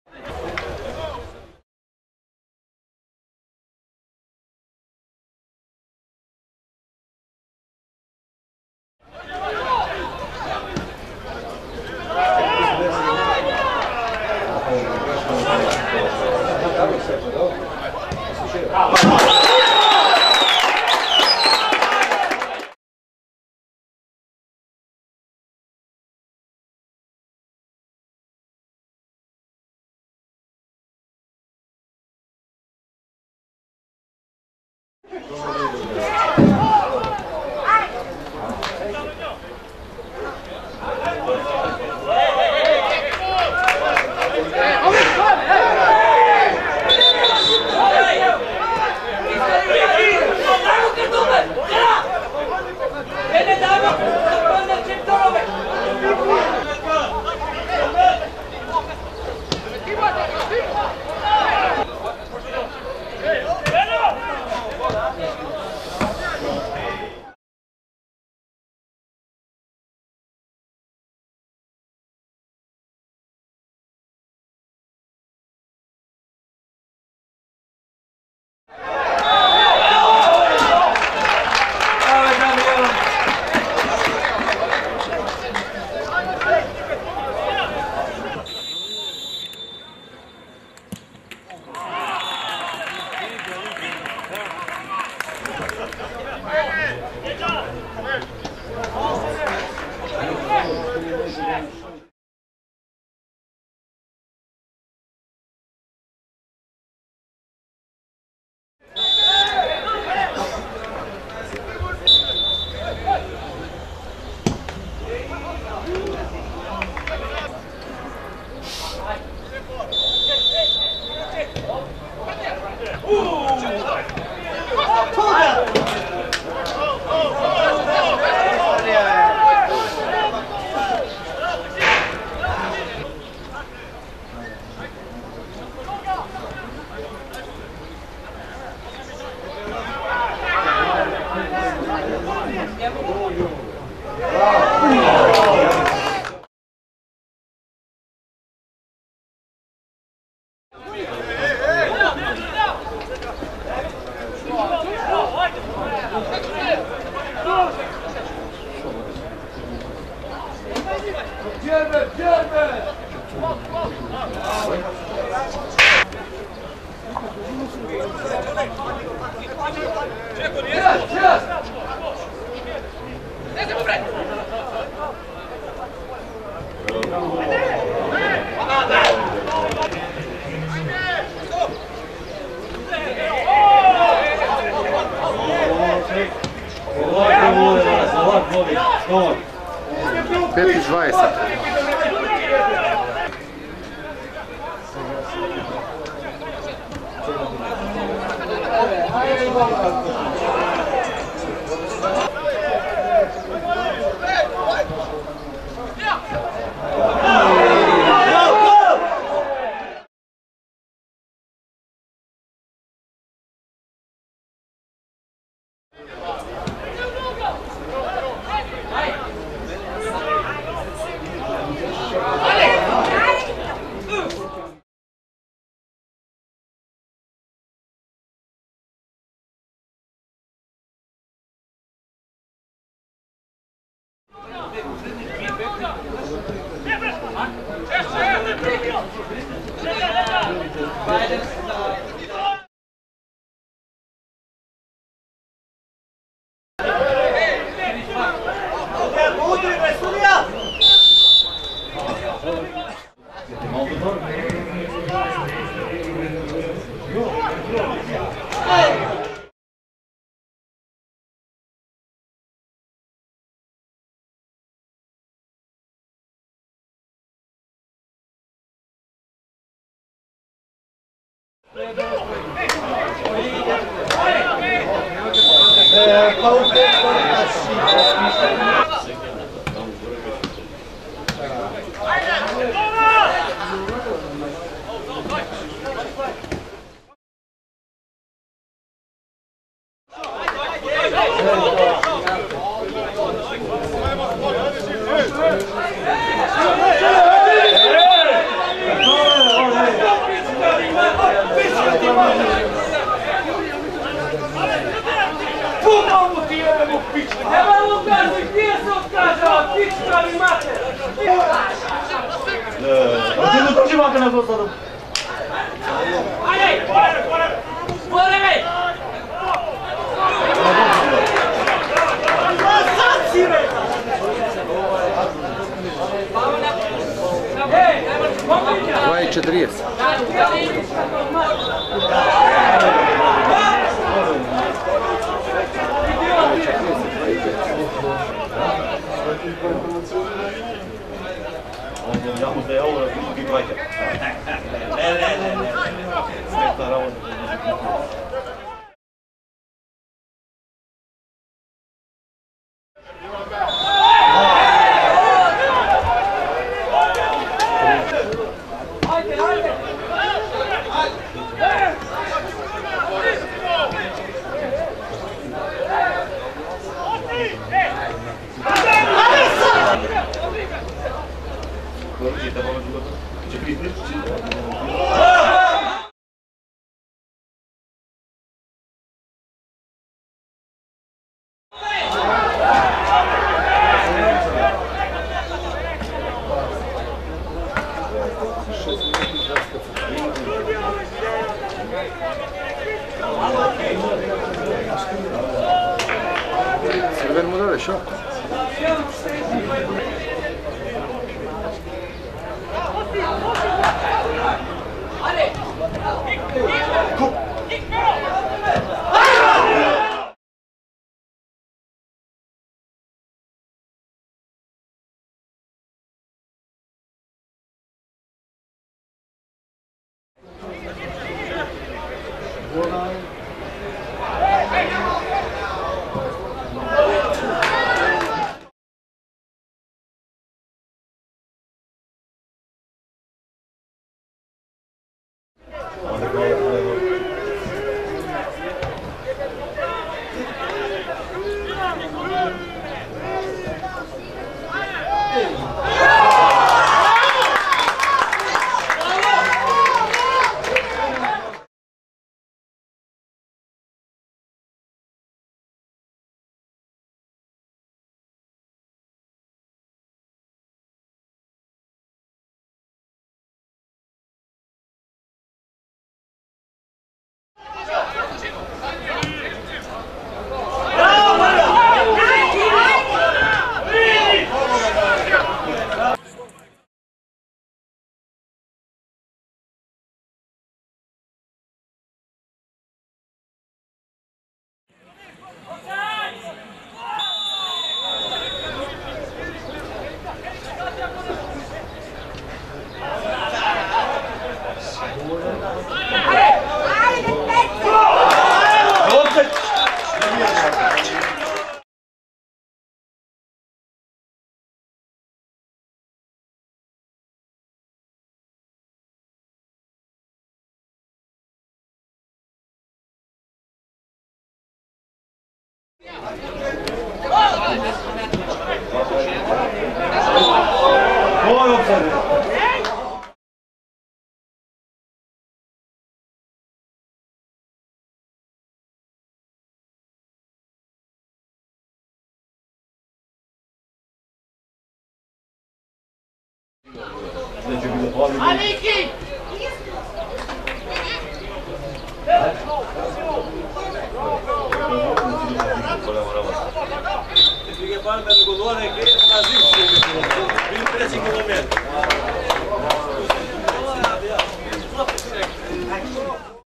FK ZAJAS - FK EUROMILK Gorno Lisiche 1:4 BARAZ 03.06.2015
SEZONA 2014/15 - BARAZ natprevar za VTORA MFL,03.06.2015
Stadion: SHTIP, HIGHLIGHTS